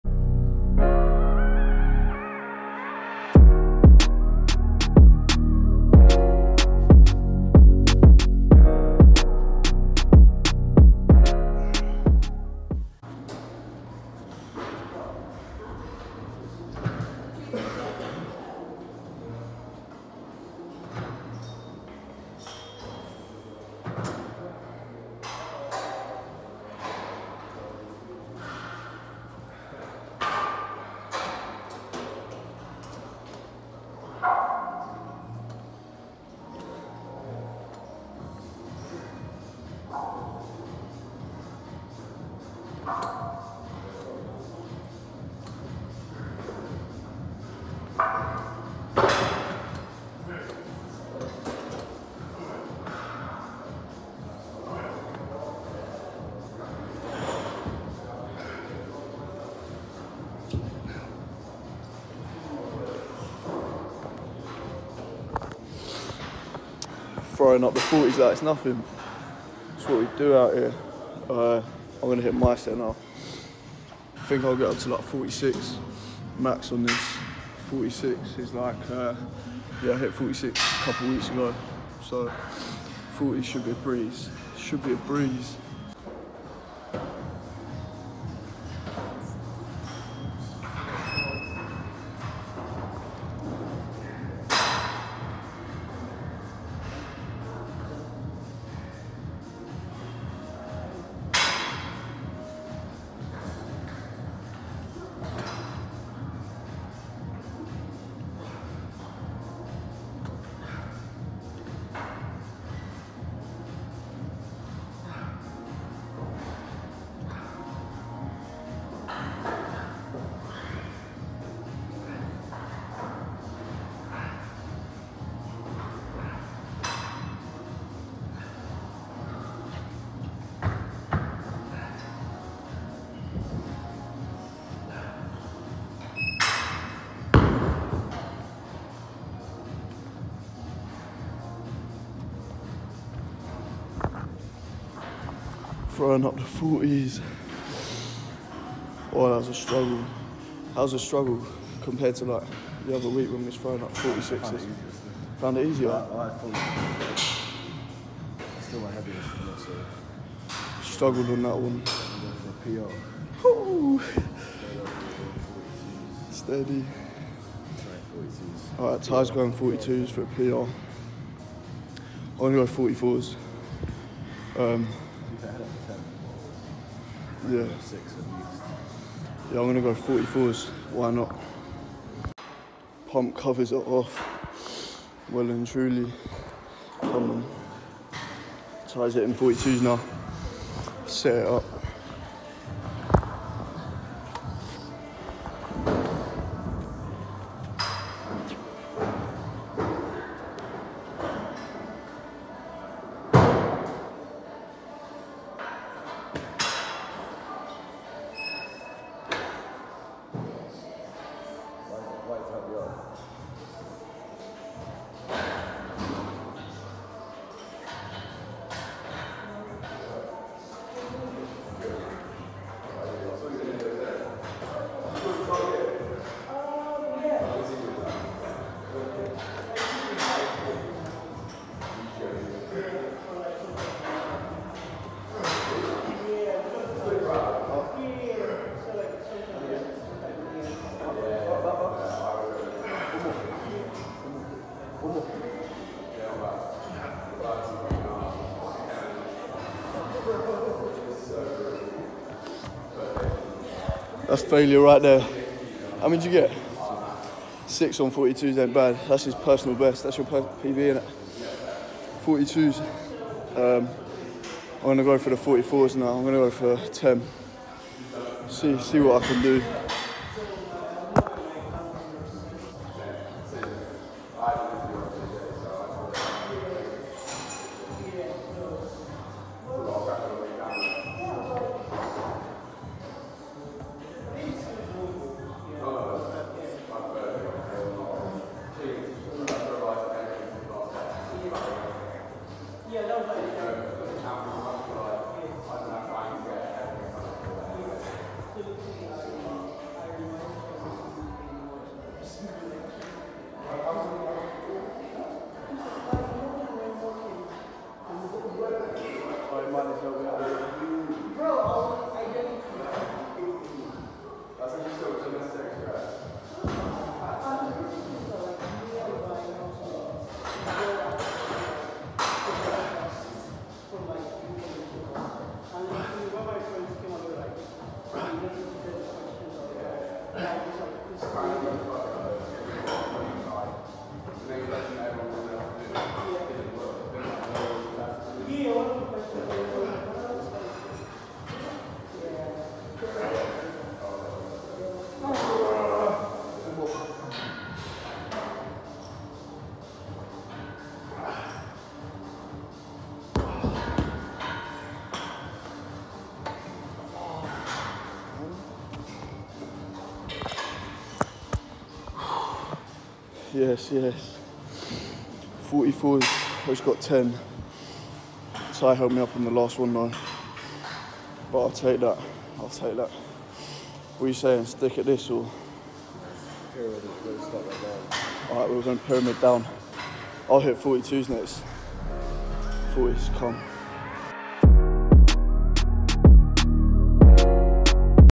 THROWING UP THE 44KG DUMBBELLS
Progressively overloading on decline chest press each week, 50s soon come.